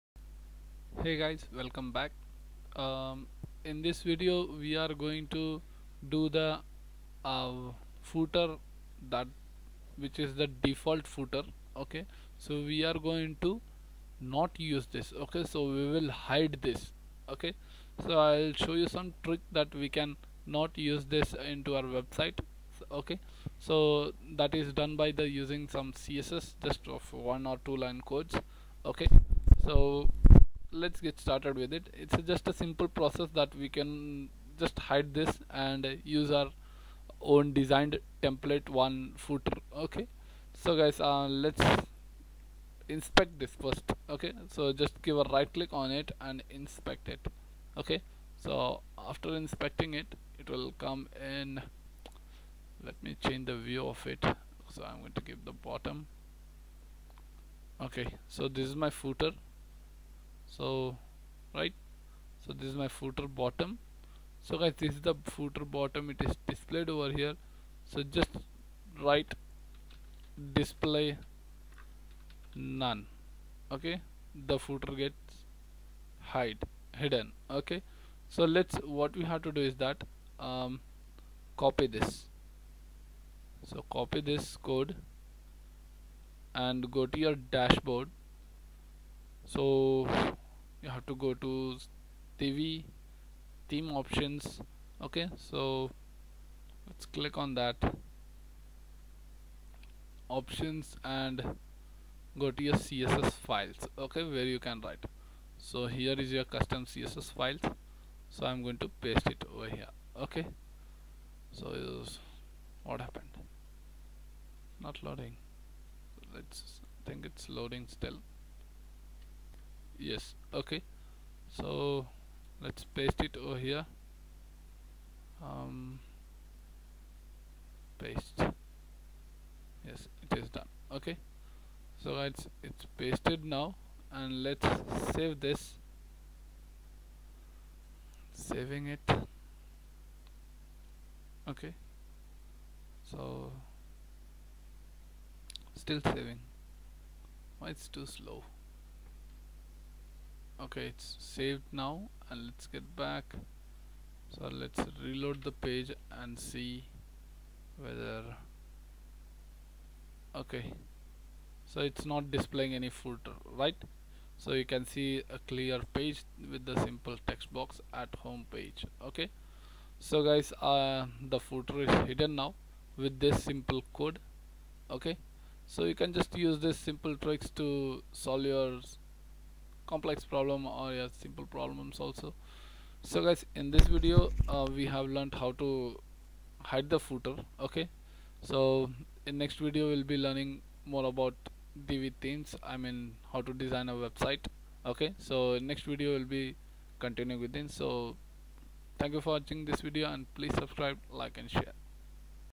How to Hide Default Footer of WordPress
Here.. in this video,, its done all about, how to hide footer or remove the footer by CSS in WordPress.